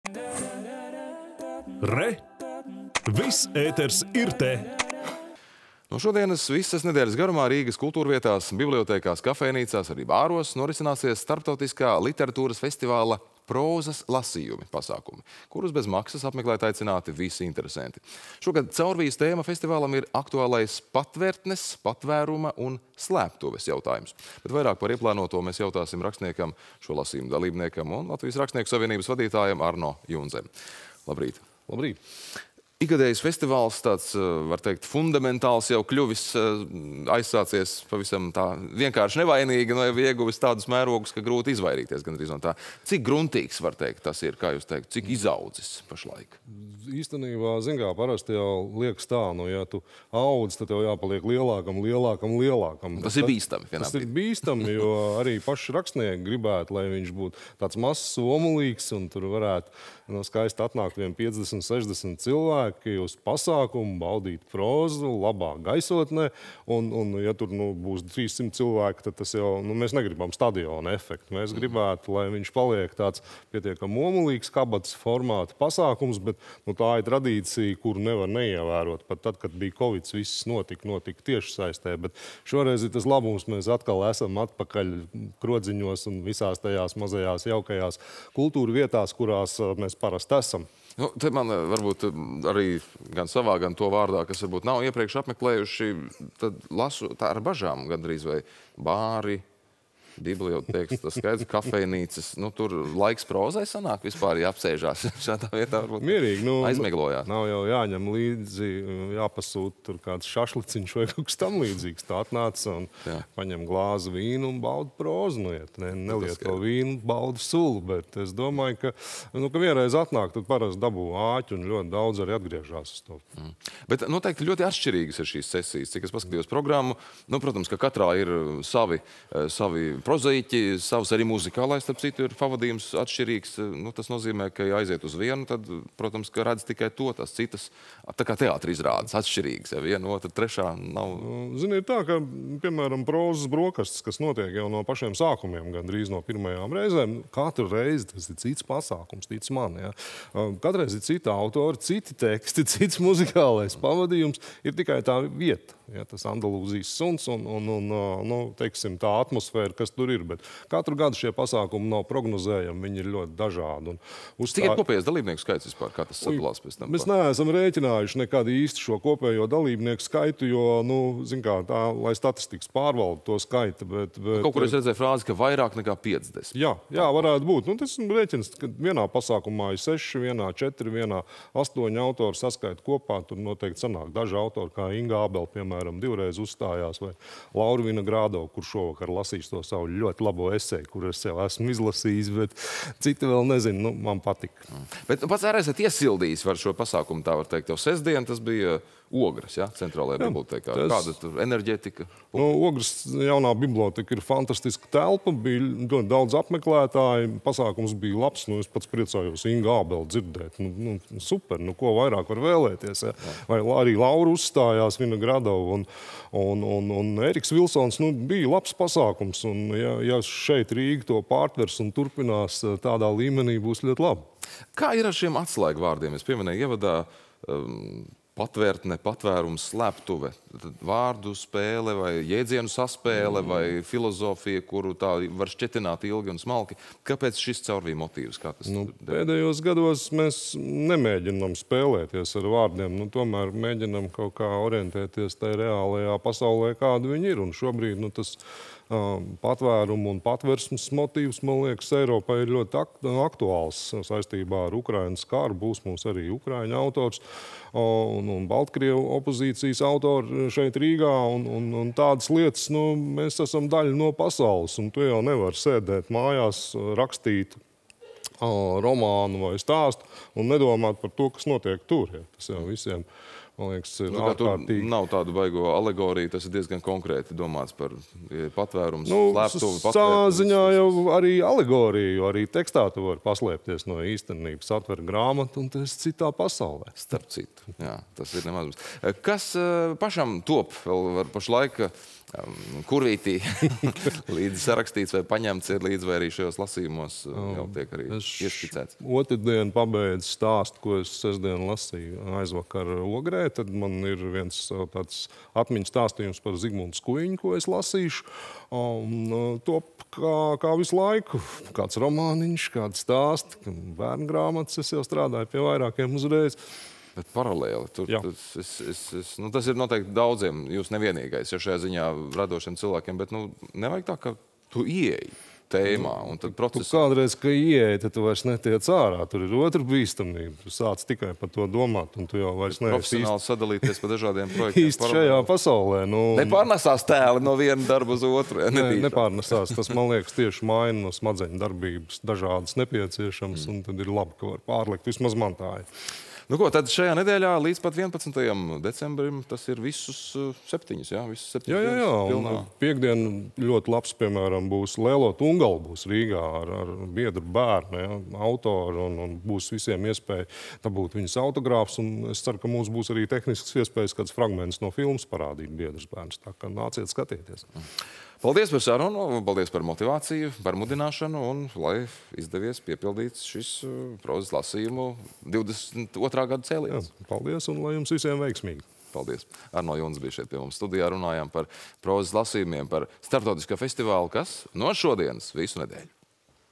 Starptautiskais literatūras festivāls “Prozas lasījumi 2022”
šodienas visas nedēļas garumā (līdz 11. Decembrim)  Rīgas kultūrvietās, bibliotēkās, kafejnīcās un bāros norisināsies starptautiskā literatūras festivāla “Prozas lasījumi 2022” pasākumi, kurus bez maksas apmeklēt aicināti visi interesenti.
     Šogad caurvijas tēma festivālam ir aktuālais patvertnes, patvēruma un slēptuves jautājums. Vairāk par ieplānoto jautāsim rakstniekam, šo lasījumu dalībniekam un Latvijas Rakstnieku savienības vadītājam Arno Jundzem.